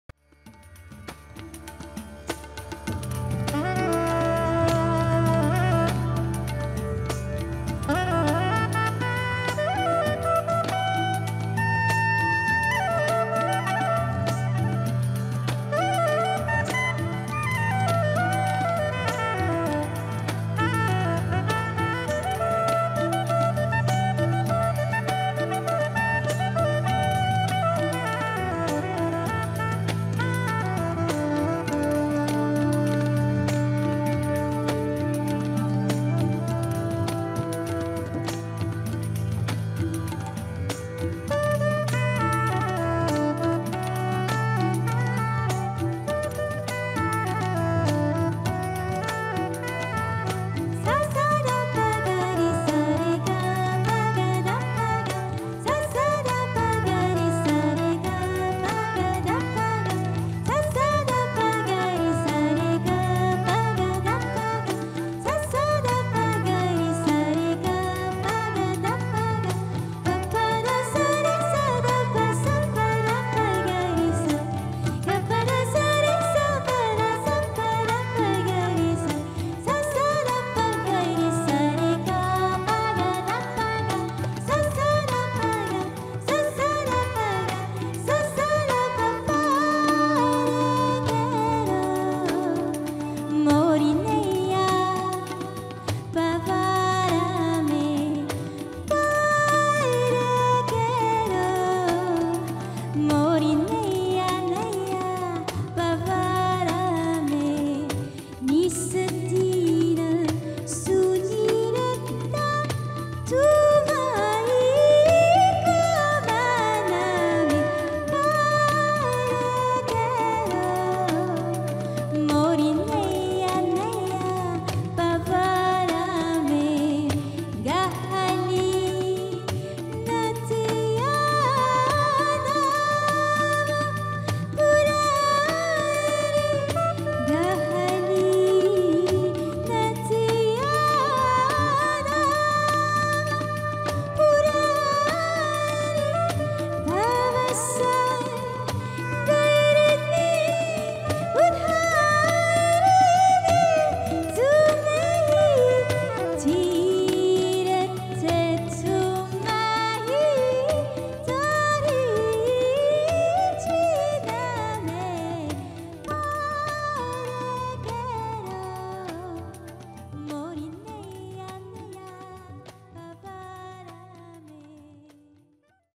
BollyBhoop Escoffier1
Chaar Masala et la compagnie "Indian Dance Navrasa" à la salle Escoffier à Villeneuve Loubet